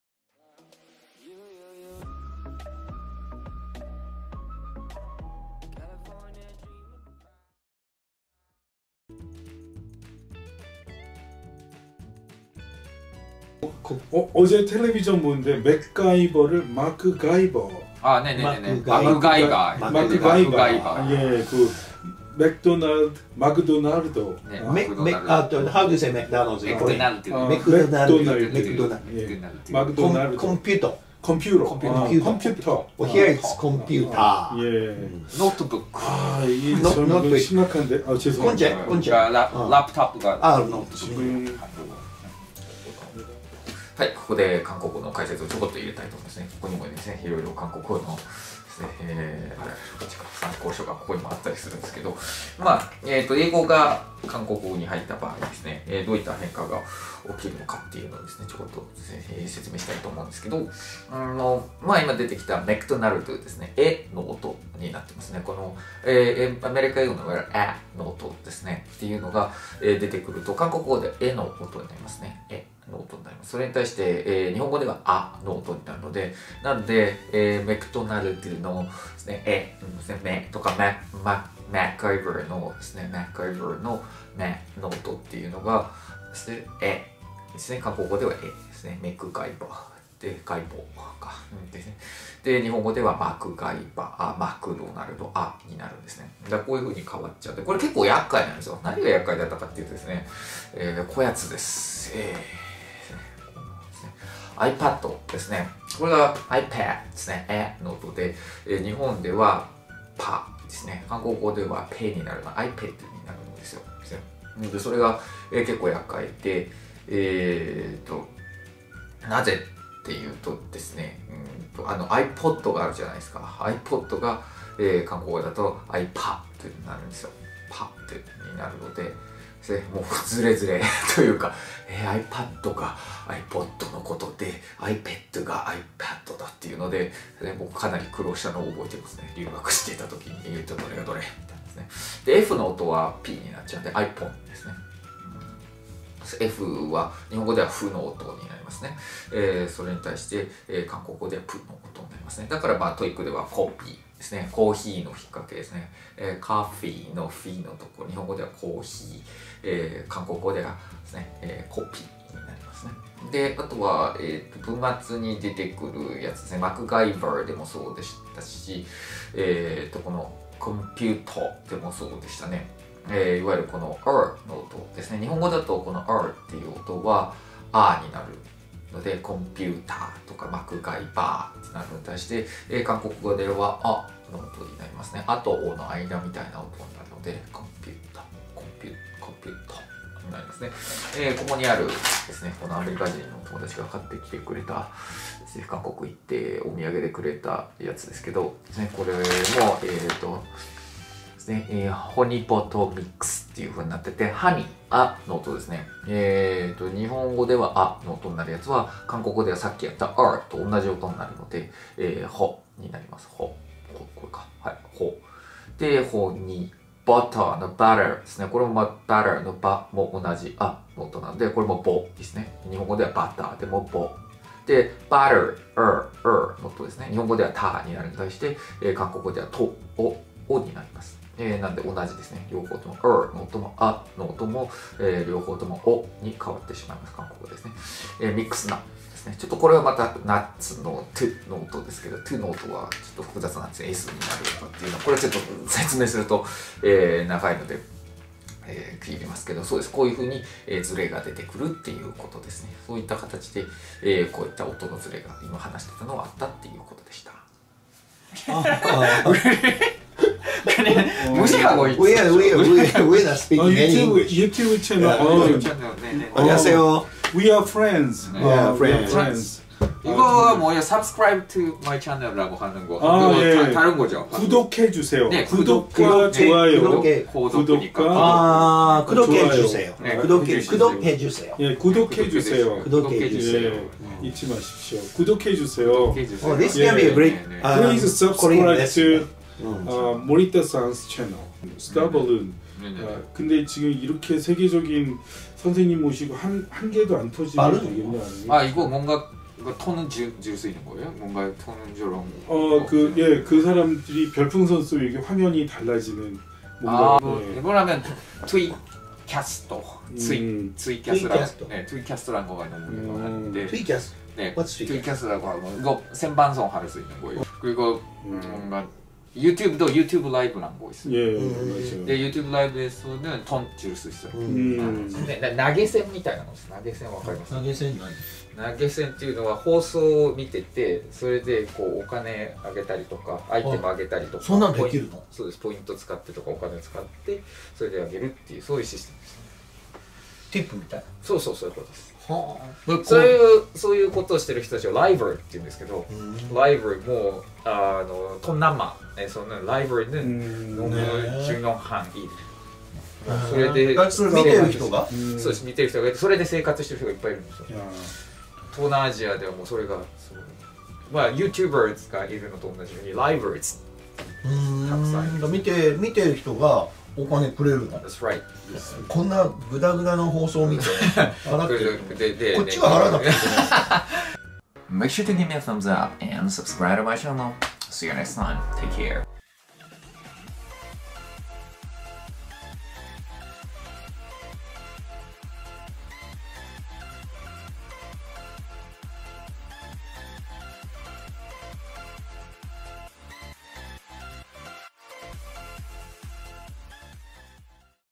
韓国語英語ー英語が韓国語になると日本語とどう発音が変わる？
英語が韓国語に入ると日本語とどう変わるのか
マクドナルドは？コンピューターは？発音のルールを解説します。